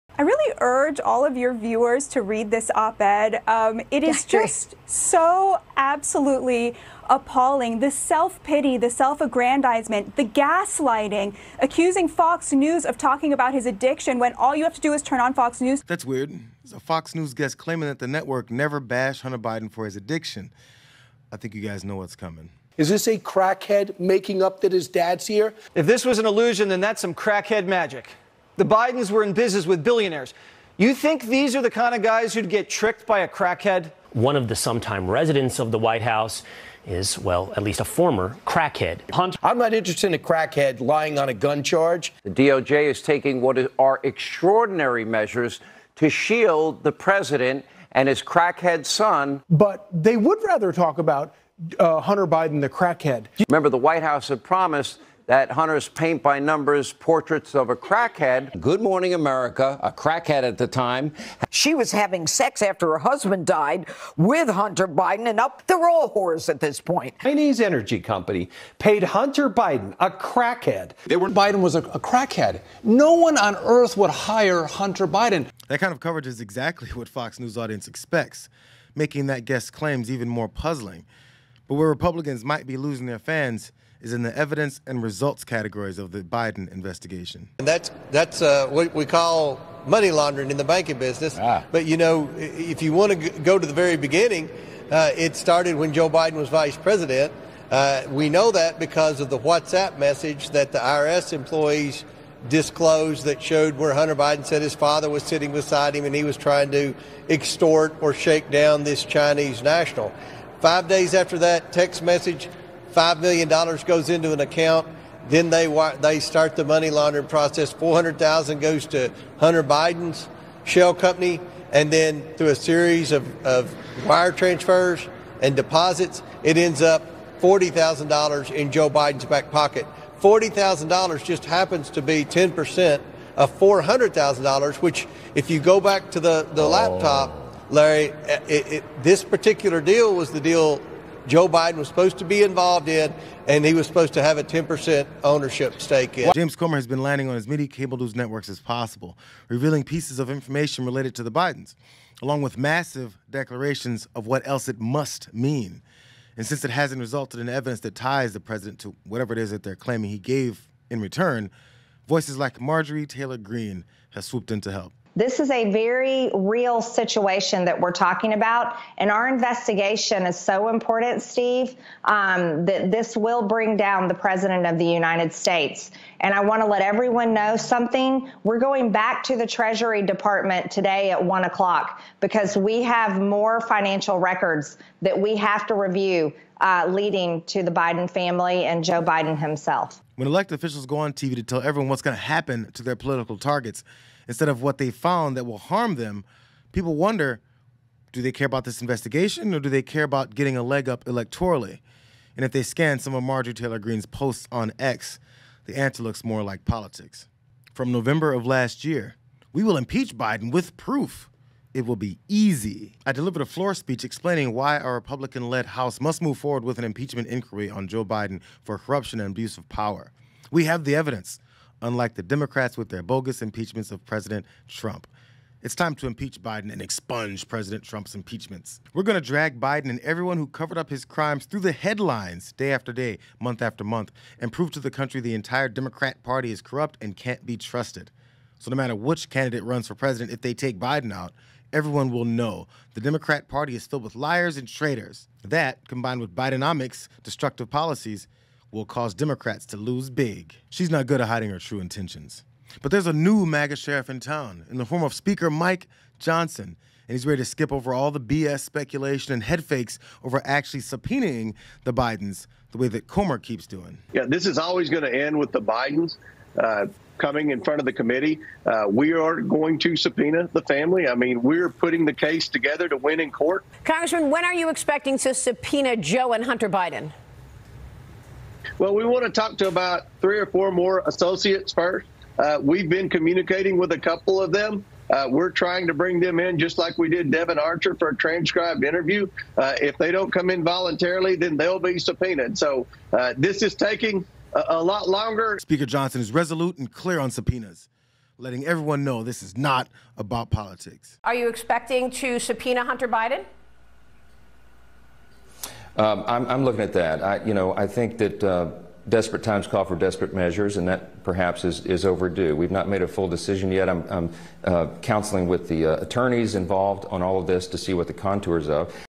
Fox News Guest Gets DEBUNKED By Network's Own Comments
Journalist Batya Ungar-Sargon's assertion that Fox News refrains from ridiculing Hunter Biden for his addiction is contradicted by numerous instances of the network's hosts insulting Hunter Biden. Jayar Jackson breaks it down.